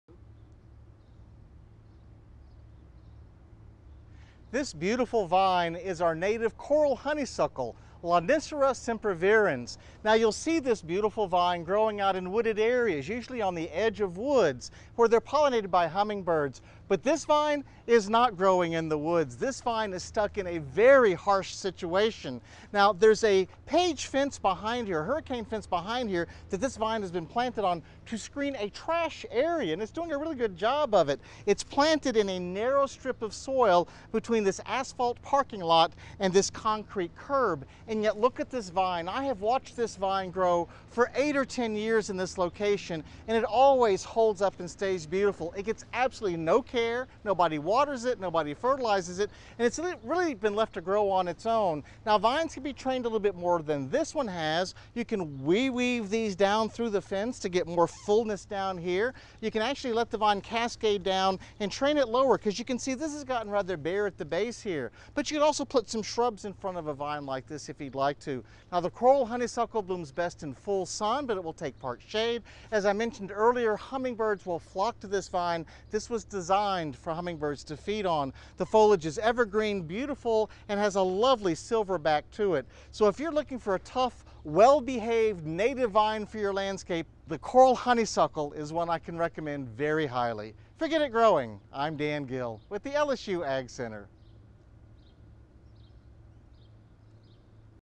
Showy coral honeysuckle attracts hummingbirds
The coral honeysuckle is one of the showiest vining honeysuckles you can find. It's tough and attracts both hummingbirds and butterflies. On this edition of Get It Growing, LSU AgCenter horticulturist Dan Gill explains some uses for this climbing vine with bright flowers.